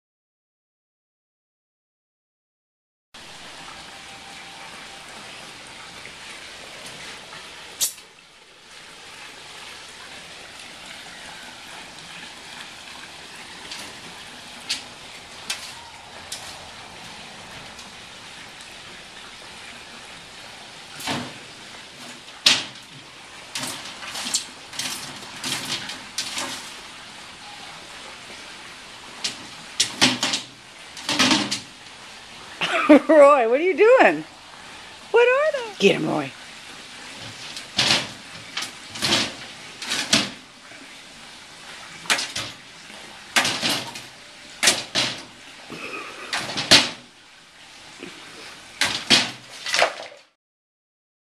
Baby Boi & his little friends.mp4
My afghan hound just notices my son's cricket cage.